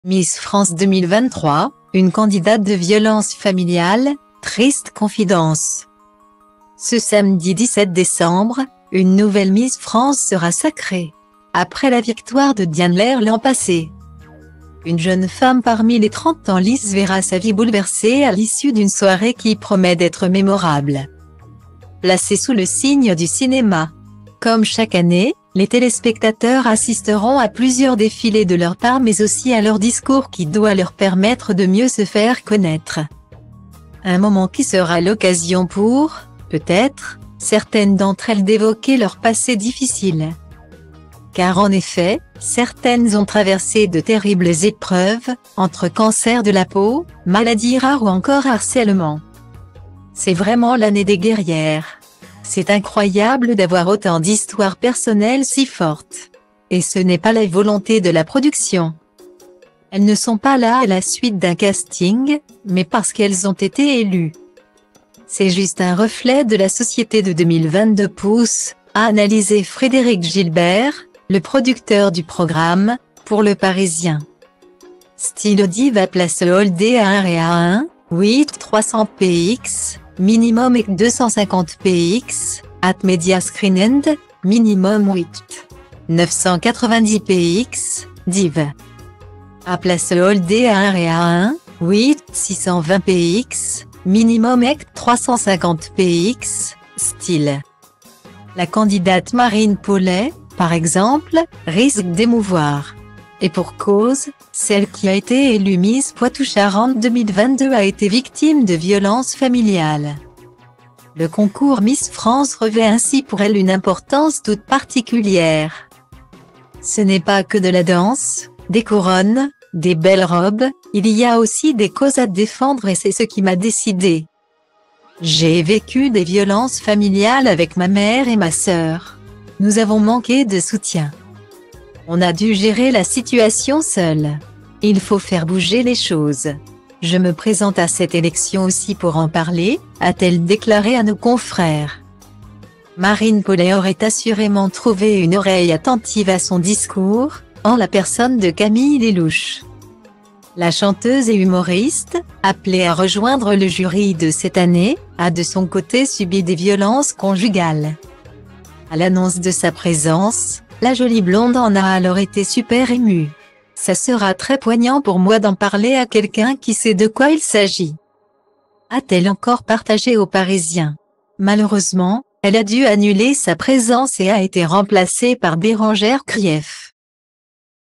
Miss France 2023 : Une candidate de violences familiales, tristes confidences
Ce samedi 17 décembre, une nouvelle Miss France sera sacrée ! Après la victoire de Diane Leyre l'an passé,une jeune femme parmi les 30 en lice verra sa vie bouleversée à l'issue d'une soirée qui promet d'être mémorable,placée sous le signe du cinéma. Comme chaque année, les téléspectateurs assisteront à plusieurs défilés de leur part mais aussi à leur discours qui doit leur permettre de mieux se faire connaître.